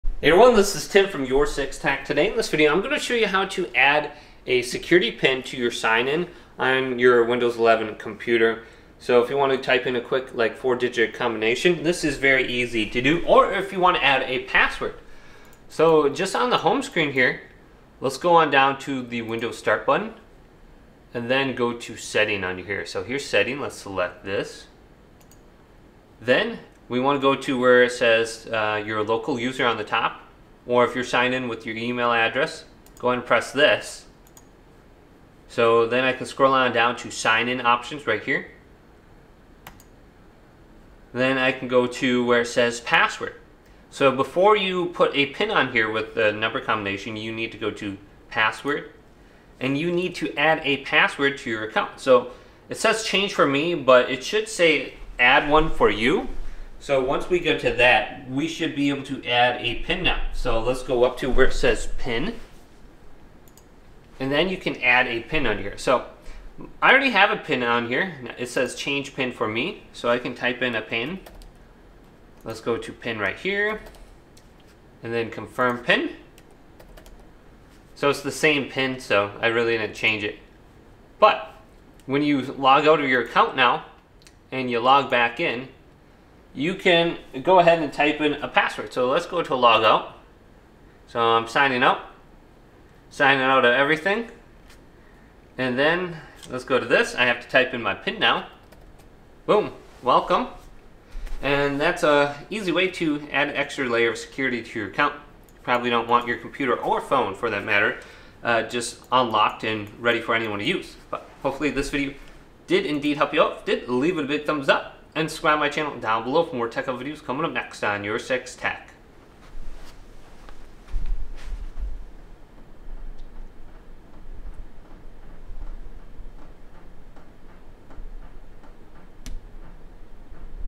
How to Add a Security PIN to LOGIN on Account for Windows 11 (4 Digit Code)
Adding a PIN for secure login in Windows 11 is straightforward. Follow these steps:

Open Settings:
Press the Windows key and select Settings from the Start menu.
Alternatively, use the “Windows key + I” shortcut or right-click on the Start menu and choose “Settings”.
Navigate to Accounts:

Click on “Accounts” in the left column.
Access Sign-in Options:
Under “Account settings,” select “Sign-in options” on the right side.
Set Up Your PIN:

Expand the “PIN (Windows Hello)” option.
Click the “Set up” button.
Authenticate by entering your account password.
Create Your PIN:
Type a numeric PIN in both fields.
Click “OK” to complete the setup.